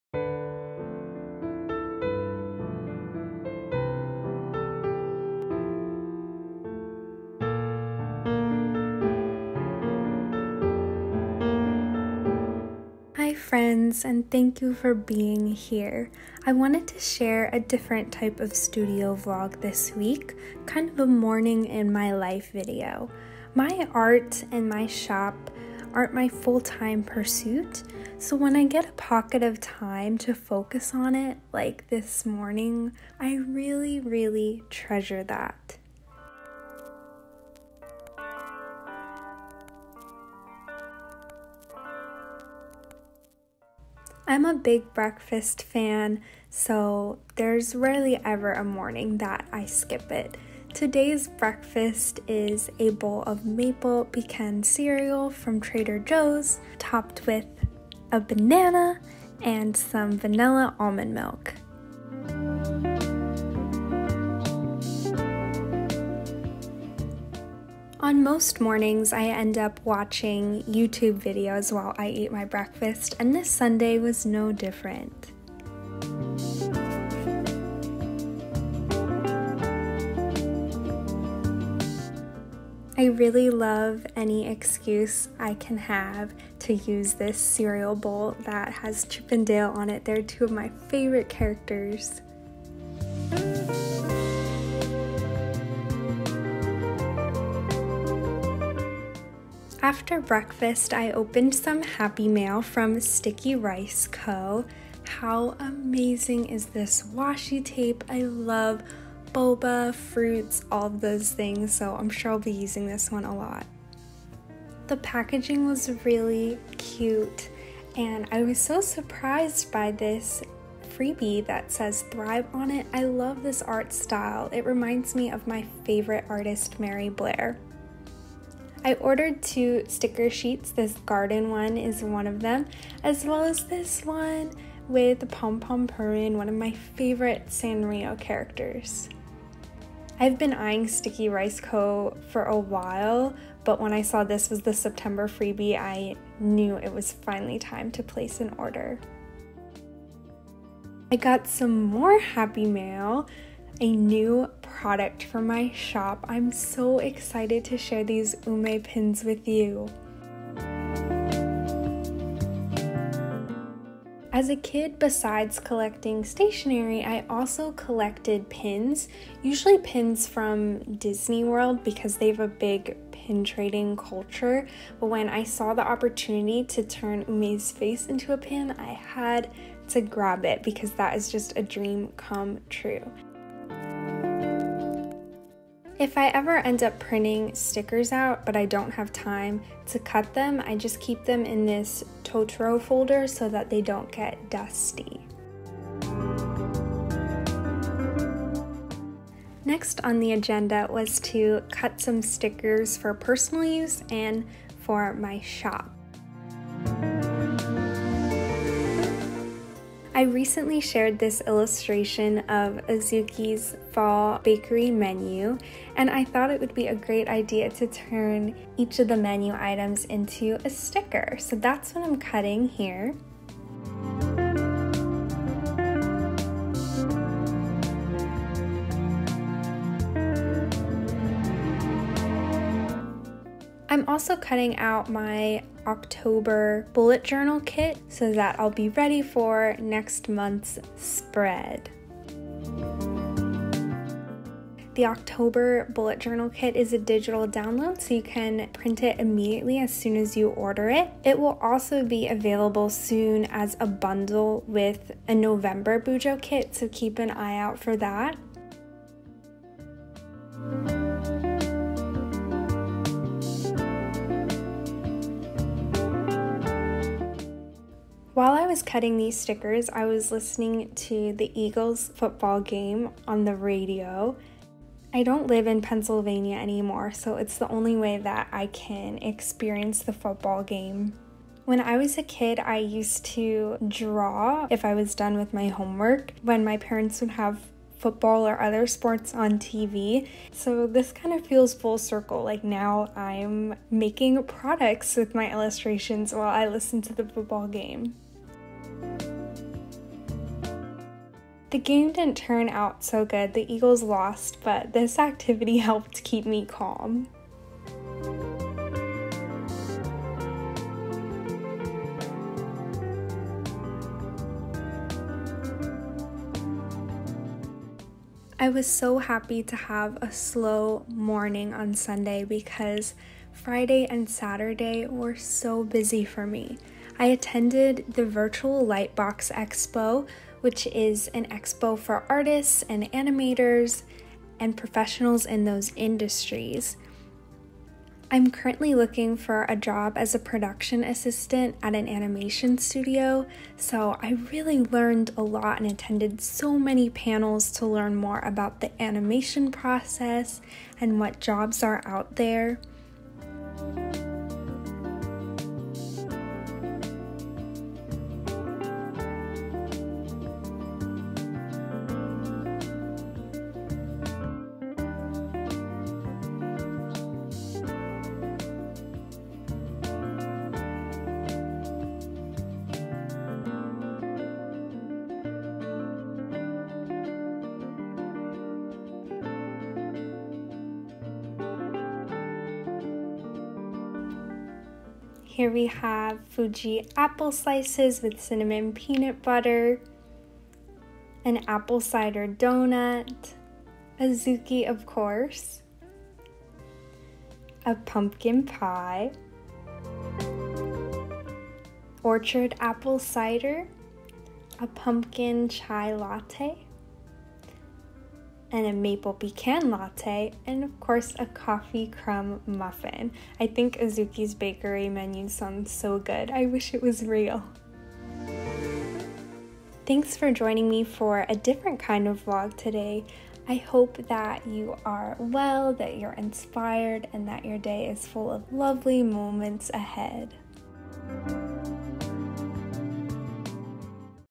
studio vlog 5 ~ a morning in my life as an etsy sticker shop owner
thank you for being here! I hope you enjoy this different type of studio vlog.

a big shop update is coming next week on 9/22, so make sure you’re following me on instagram and that you’ve favorited my shop on etsy to find out when it’s live!